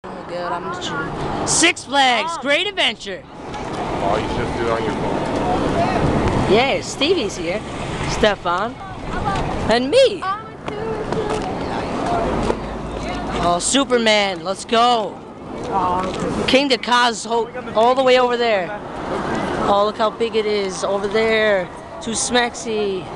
Six Flags With Stefman398 Crew vlog
At Six Flags and went on all the good rides with my friends on our youtube channel.